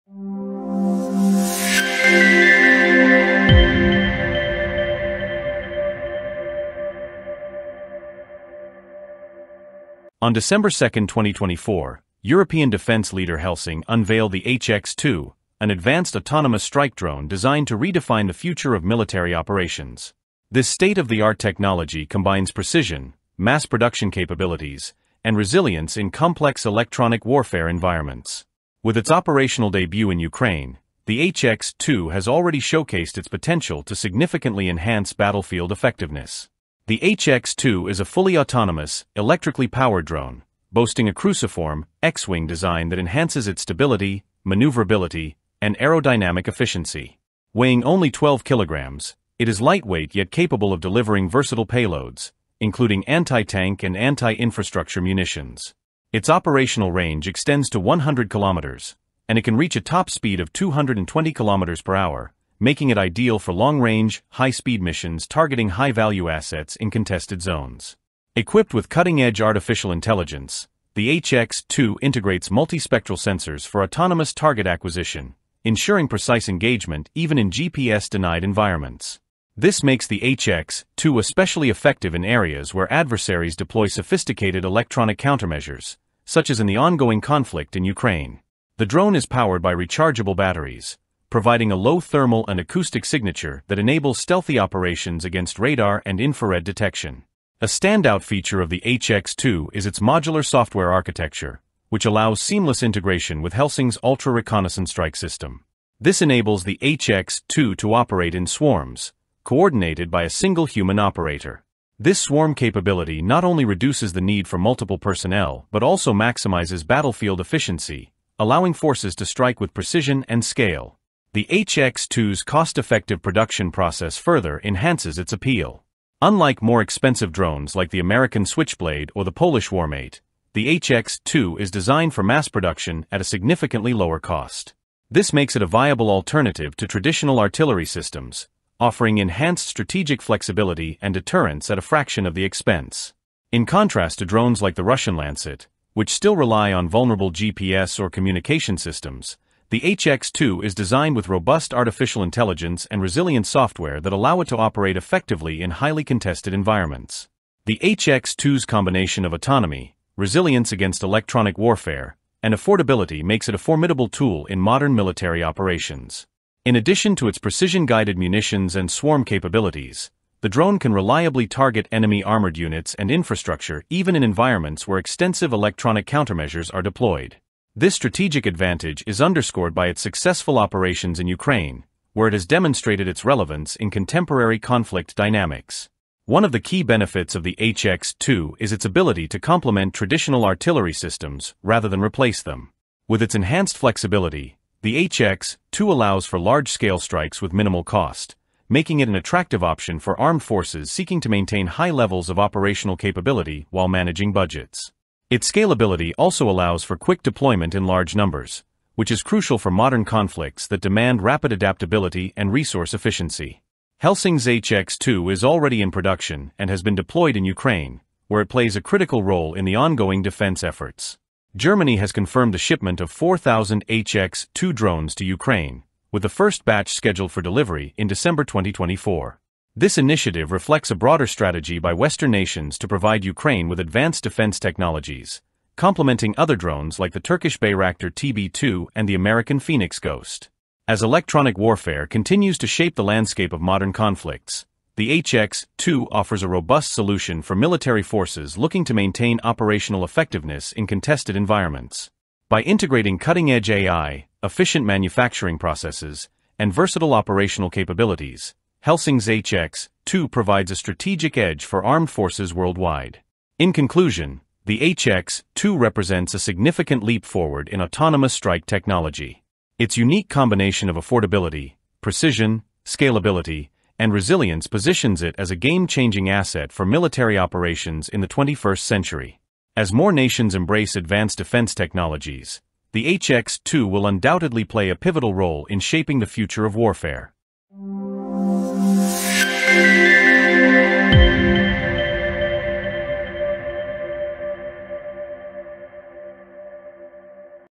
Helsing Launches HX 2 Autonomous Strike Drone with Proven Success in Ukraine
The HX-2 autonomous strike drone by Helsing is a groundbreaking innovation in modern military technology. Featuring full autonomy, electronic warfare resilience, and cost-efficient mass production, it is ideal for high-value operations in contested environments. The drone's lightweight design, 100 km range, and stealth capabilities make it a powerful tool for anti-tank and anti-infrastructure missions. Powered by AI, the HX-2 utilizes advanced sensors and algorithms to operate even in GPS-denied zones. With integration into Helsing’s Altra system, it enables efficient swarm operations, providing unmatched precision and flexibility. Already deployed in Ukraine, the HX-2 sets new standards for modern warfare technology.